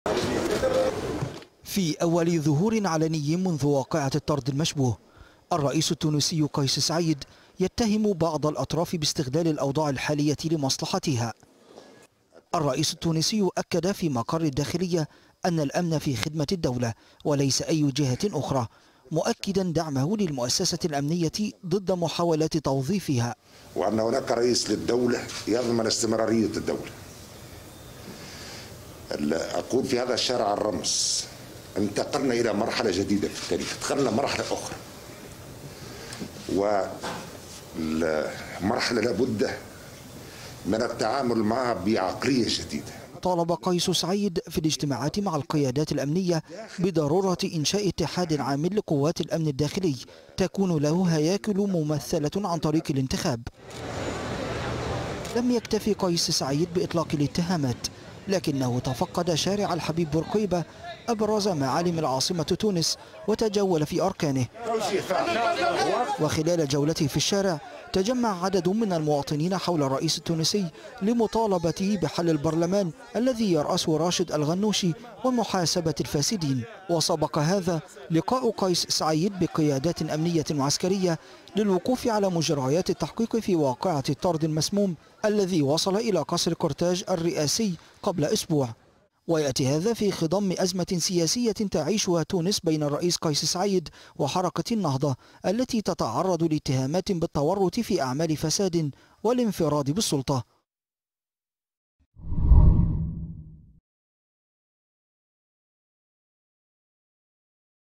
باتهامات لبعض الجهات التونسية.. أول تعليق رسمي من قيس سعيد على محاولة اغتياله
اتهم الرئيس قيس سعيد بعض الأطراف بمحاولة استغلال الأوضاع في البلاد خاصة الأمنية لمصلحتها ، رافضا اي محاولات لتوظيفها.

وخلال لقاء في مقر وزارة الداخلية مع القيادات الأمنية، أكد قيس سعيد أن الأمن في خدمة الدولة وليس أي جهة أخرى.